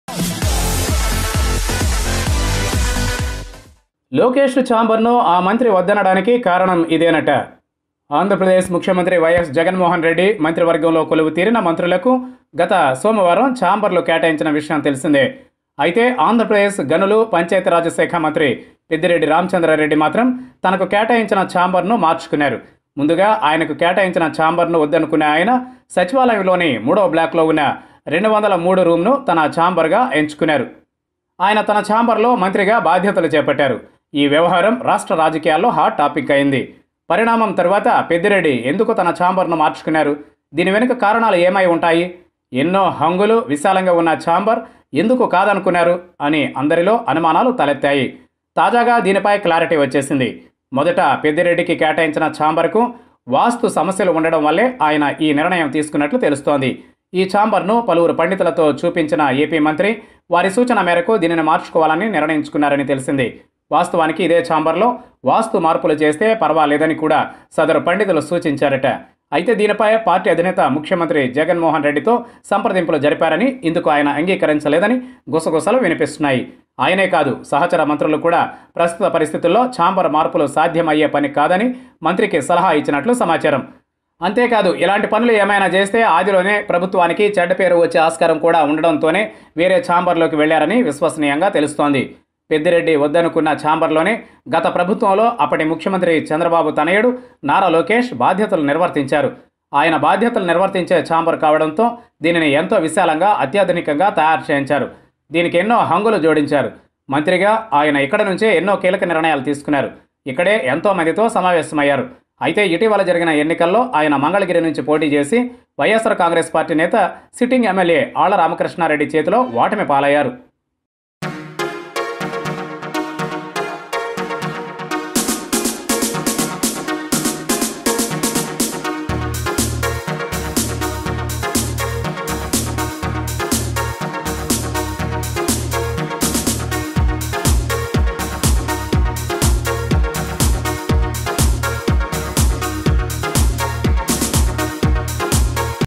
లోకేష్ ఛాంబర్ పేరు చెబితే భయపడిన పెద్దిరెడ్డి|Nara Lokesh Chamber Rejected by Minister Peddireddy..!
AP Panchayati Raj Minister Peddireddy Ramachandra Reddy was allotted the chamber used by Nara Lokesh while he served as an IT and Panchayati Raj Minister. The new Minister, however, refused to move it to the chamber of Former CM's Son. Later, He was allotted Room No.203 in 3rd Block of AP Secretariat and the Senior Politician accepted it happily. What could be the reason behind rejecting Lokesh's Chamber which is spacious and equipped with all the facilities?
Watch this video to know the reason behind rejecting Lokesh's Chamber
Amaravathi Media